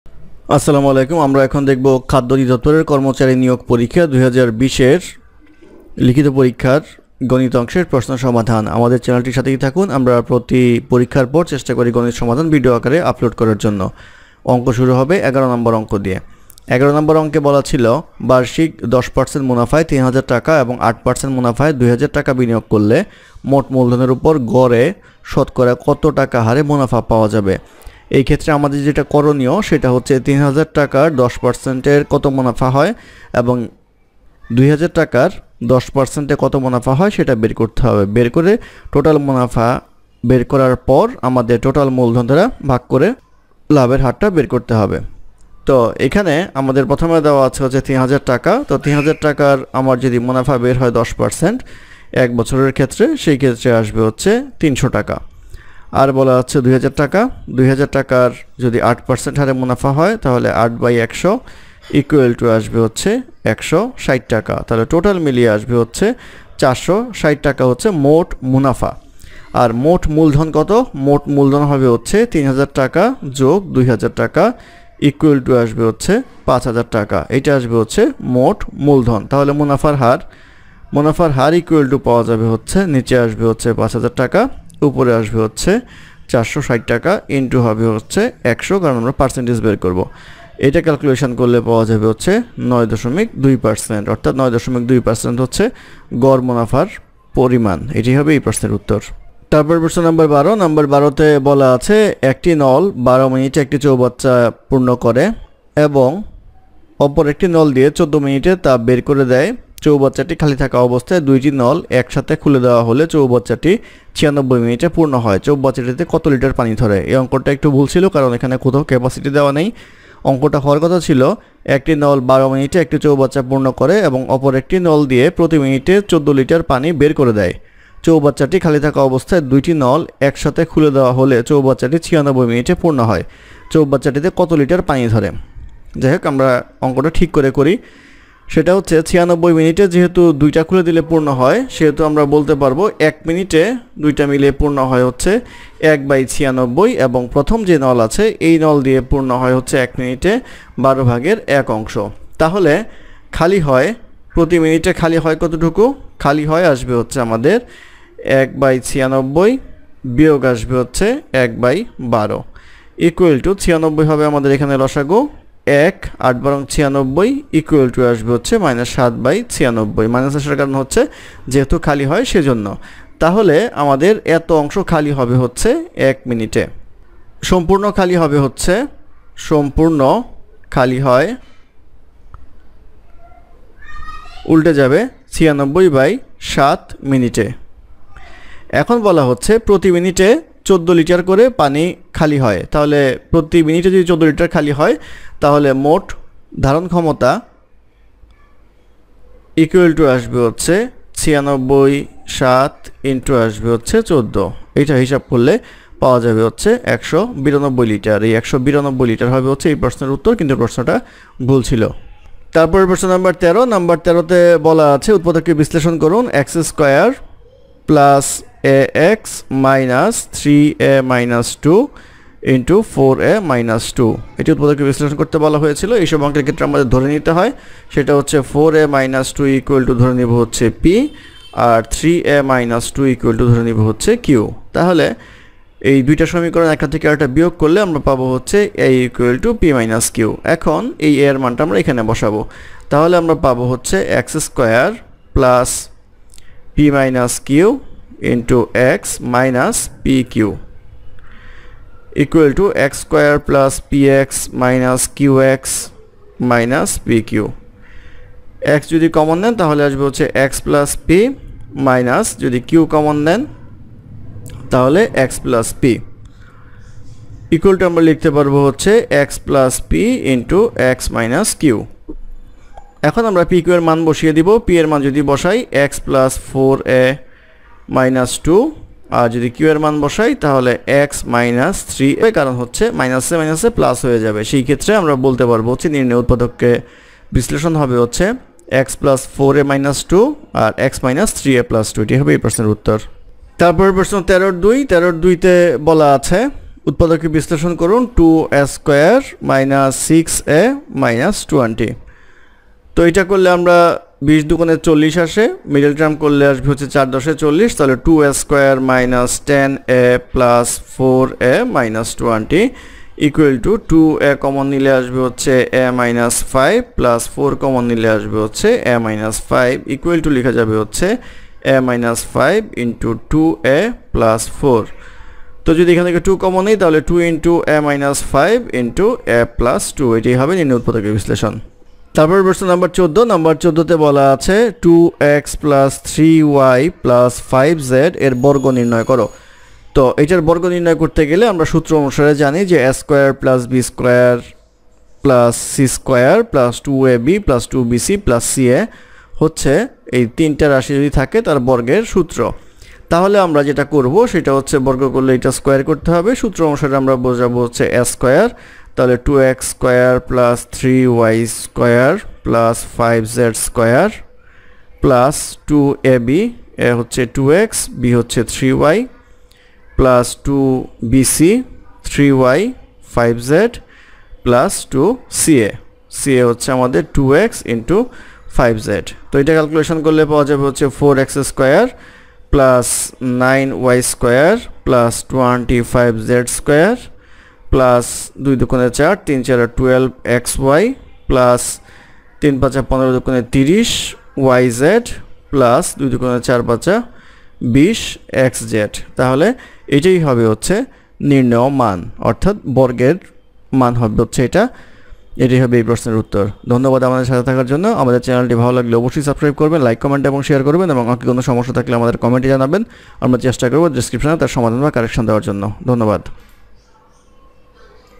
খাদ্য অধিদপ্তর পদ: বিভিন্ন পদ (কর্মচারী) Written Math Solution Exam Date: 31-10-2020
mathhelpline mathhelpline খাদ্য অধিদপ্তর পদ: বিভিন্ন পদ (কর্মচারী)

১১.বার্ষিক ১০% মুনাফায় ৩০০০ টাকা এবং ৮% মুনাফায় ২০০০ টাকা বিনিয়োগ করলে মোট মূলধনের উপর গড়ে শতকরা কত টাকা হারে মুনাফা পাওয়া যাবে?
১২.একটি নল ১২ মিনিটে একটি চৌবাচ্চা পূর্ণ করে এবং অপর একটি নল দিয়ে ১৪ মিনিটে তা বের করে দেয় ( অপর একটি নল দিয়ে প্রতি মিনিটে ১৪ লিটার পানি বের করে দেয়) চৌবাচ্চাটি খালি থাকা অবস্থায় দুইটি নল একসাথে খুলে দেওয়া হলে চৌবাচ্চাটি ৯৬ মিনিটে পূর্ণ হয়। চৌবাচ্চাটিতে কত লিটার পানি ধরে?
১৩. উৎপাদকে বিশ্লেষণ করুন: (i) x2+ax-(3a-2)(4a-2)  (ii) 2a2-6a-20
১৪. 2x+3y+5z এর বর্গ নির্ণয় করুন